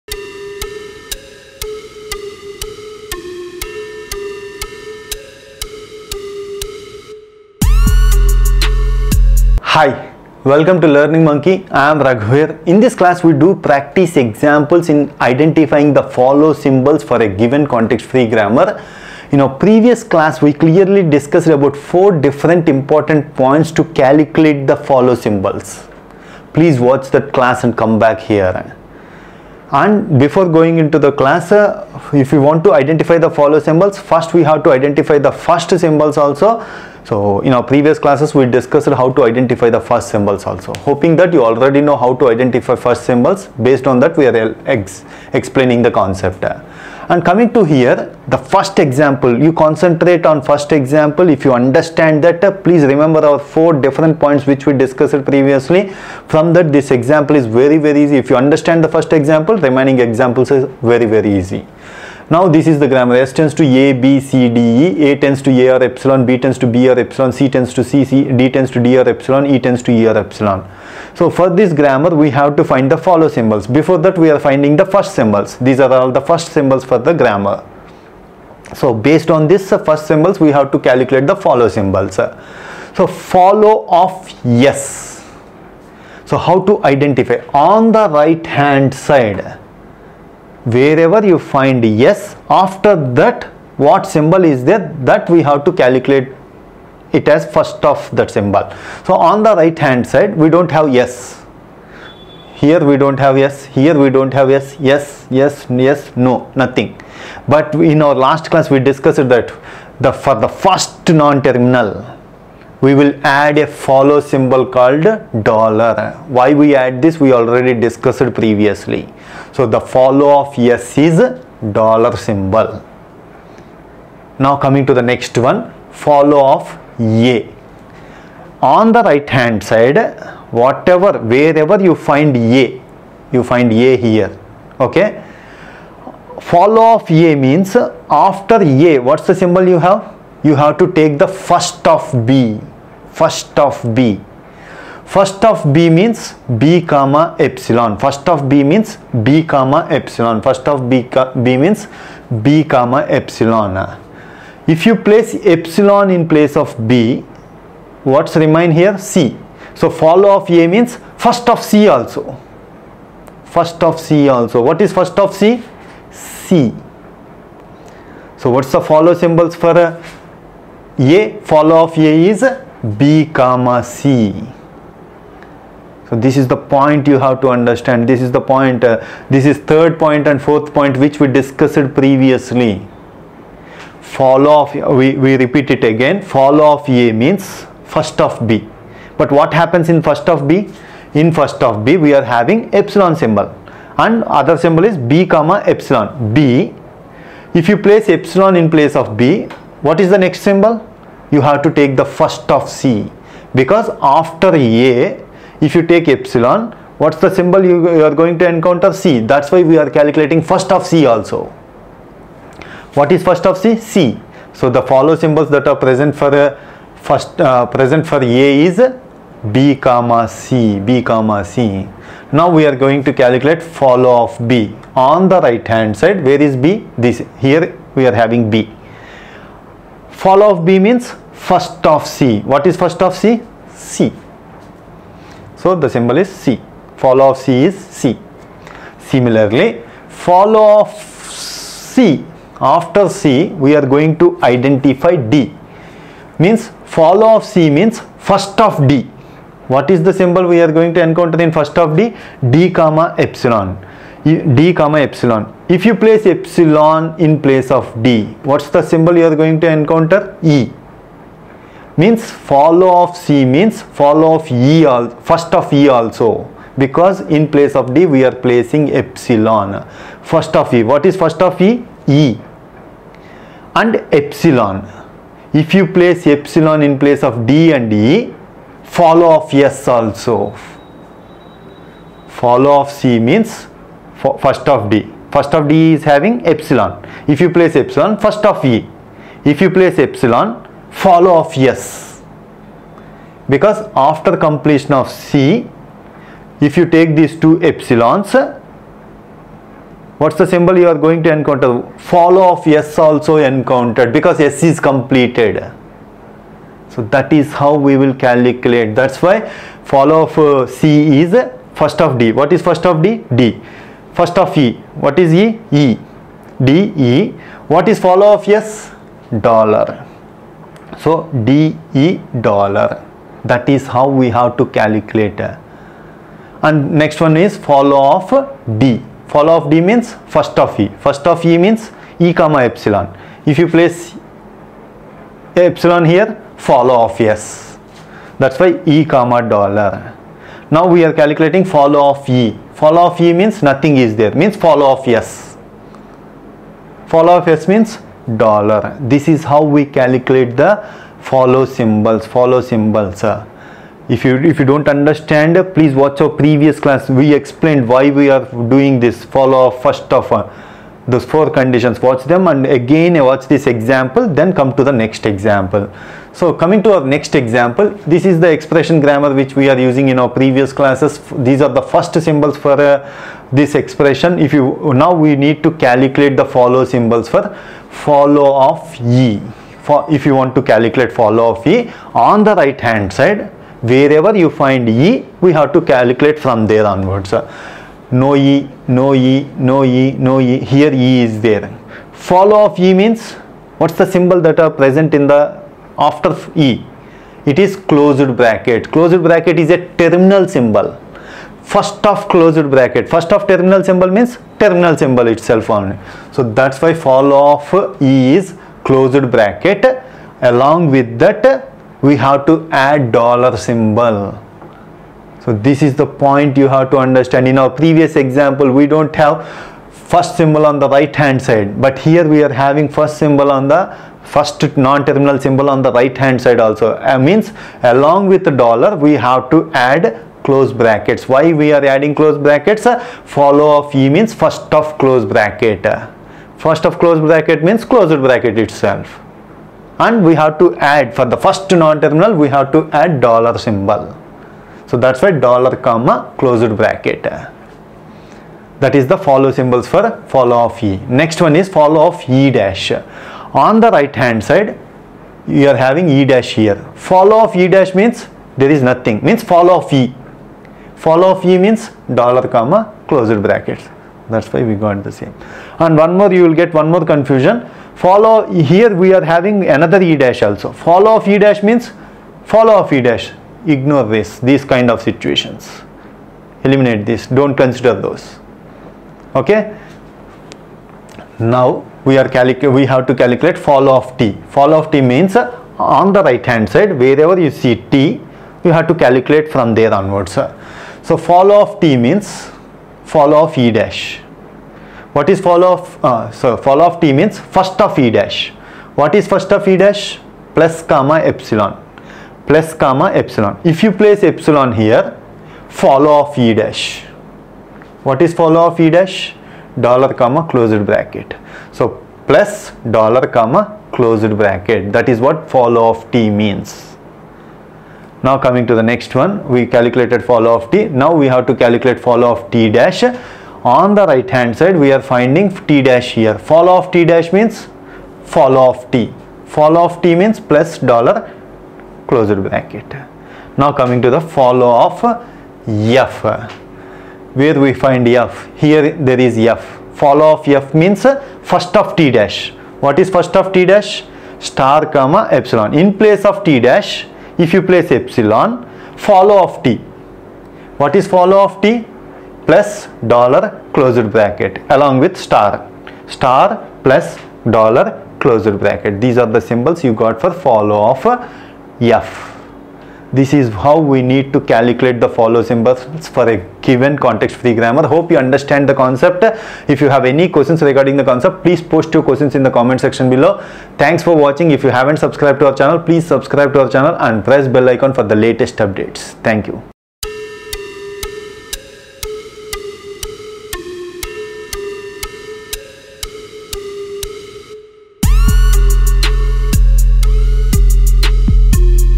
Follow Examples in Compiler || Lesson 21 || Compiler Design || Learning Monkey
Follow Examples in Compiler
In this class, We discuss Follow Examples in Compiler.
The reader should have prior knowledge of 'follow' conditions. Click Here.
To find the 'Follow' symbols, We need to find the First symbols.
How to find the First Symbols? Click Here.
Example 1:
S – ABCDE
A – a | ε
B – b | ε
C – c
D – d | ε
E – e | ε
First Symbols for the grammar
First(S) = {a,b,c}
First(A) = {a, ε}
First(B) = {b, ε}
First(C) = {c}
First(D) = {d,ε}
First(E) = { e, ε}
Follow Symbols for the grammar
Follow(S) = {$}
For the first non-terminal, we add dollar as follow symbol.
Are there any other 'follow' symbols for S? No
If we find the symbol S on the right side of the grammar productions, we find 'follow' symbols.
In our example grammar, no production has the symbol S on the right side.
Follow(A) = First(B)
First(B) = {b, ε}
Place epsilon in place of B. We get First(C) as Follow(A).
Finally, Follow(A) = {First(B) – {ε}} U {First(C)} From our fourth condition.
Follow(A) = {b,c}
Follow(B) = First(C) = {c}
Follow(C) = {First(D) U First(E) U Follow(S) } – {ε}
First(D) had epsilon so we take First(E)
First(E) had epsilon so we take Follow(S).
Finally, remove epsilon from the 'follow' symbols.
Follow(C) = {d, e,$, ε} – {v} = {d, e, $}
Follow(D) = {e, $}
Follow(E) = {$}
Example 2:
Expression Grammar First and Follow.
E – TE’
E’ – +TE’ | ε
T – FT’
T’ – *FT’ | ε
F – id | (E)
First symbols for Expression Grammar
First(E) = First(T) = First(F) = {id, (}
First(E’) = {+, ε}v
First(T’) = {*, ε}
Follow symbols for expression grammar.
Follow(E) = {$, )}
Follow(E’) = {$, )}
Follow(T) = {+, $, )}
Follow(T’) = {+, $, )}
Follow(F) = {*, +, $, )}